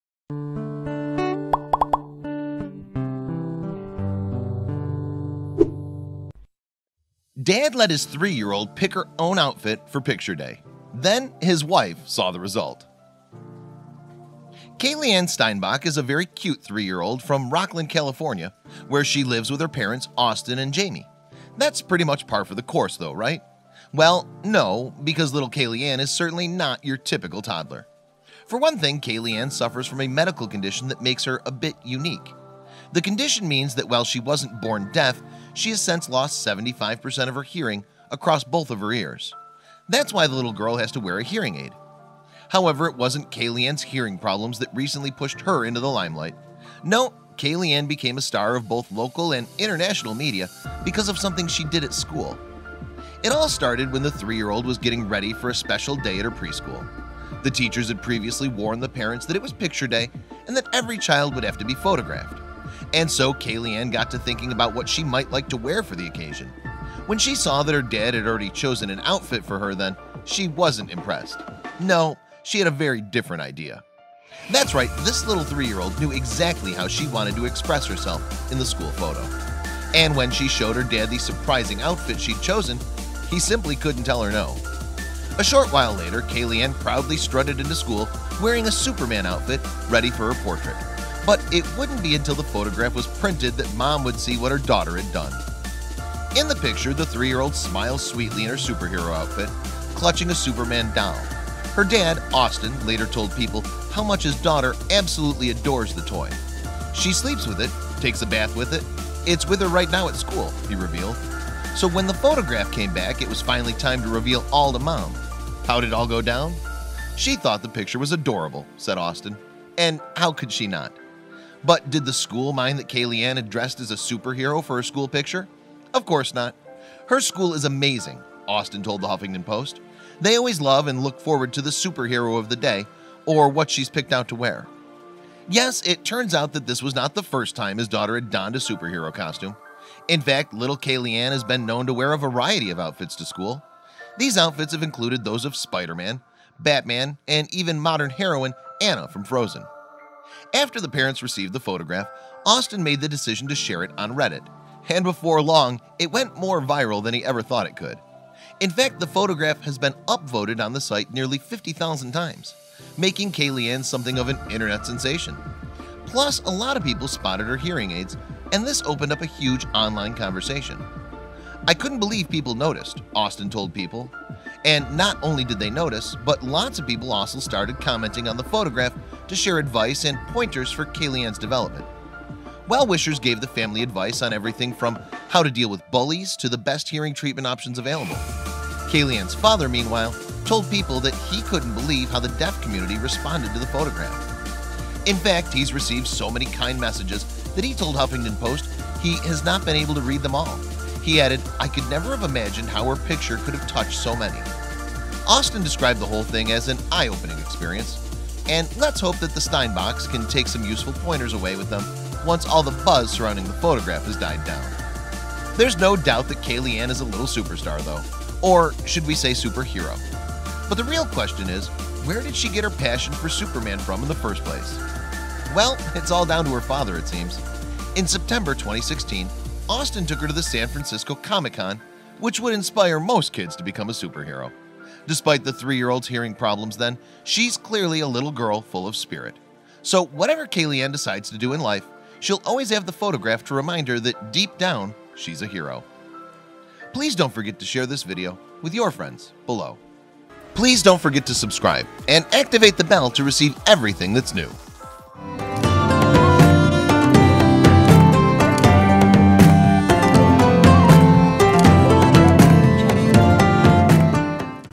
Dad Let His 3-Year-Old Pick Her Own Outfit For Picture Day. Then His Wife Saw The Result
Kaylieann Steinbach is a very cute three-year-old from Rocklin, California, where she lives with her parents, Austin and Jami. That’s pretty much par for the course, though, right? Well, no, because little Kaylieann is certainly not your typical toddler.

For one thing, Kaylieann suffers from a medical condition that makes her a bit unique. The condition means that while she wasn’t born deaf, she has since lost 75 percent of her hearing across both of her ears. That’s why the little girl has to wear a hearing aid.
However, it wasn’t Kaylieann’s hearing problems that recently pushed her into the limelight. No, Kaylieann became a star of both local and international media because of something she did at school.

It all started when the three-year-old was getting ready for a special day at her pre-school. The teachers had previously warned the parents that it was picture day and that every child would have to be photographed.

►Image credits:

Image: Facebook/Austin Steinbach
Image: Instagram/stish0715

*Thank you :Robin Lewis